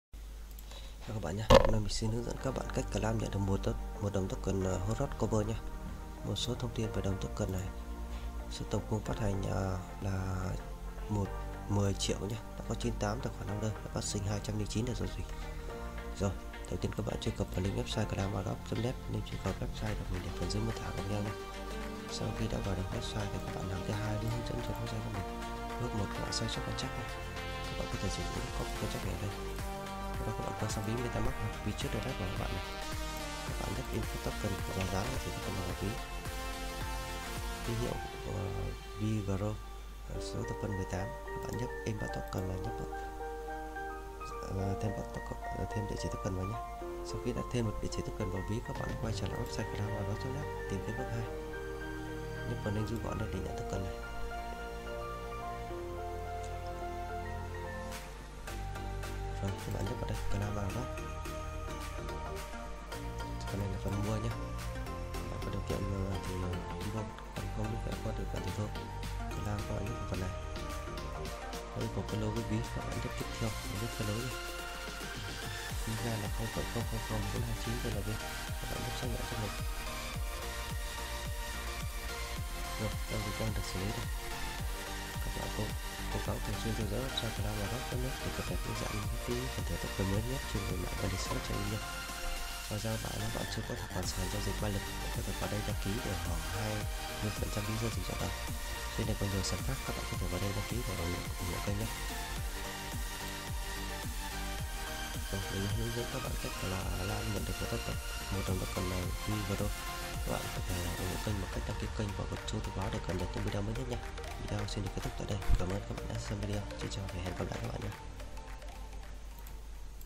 Claim Airdrop Nhận 1 Token HOROSCOPE VIRGO (VIRGO), Mạng BSC Ví Trustwallet, MetaMask
+ Bước 1: Sao chép Contract :  0x21c78796957e4fe0176d935a22394caef1cd3ccf

+ Bước 2: Truy Cập Web Tại Đây Click ( Claim Airdrop ) Nhận Token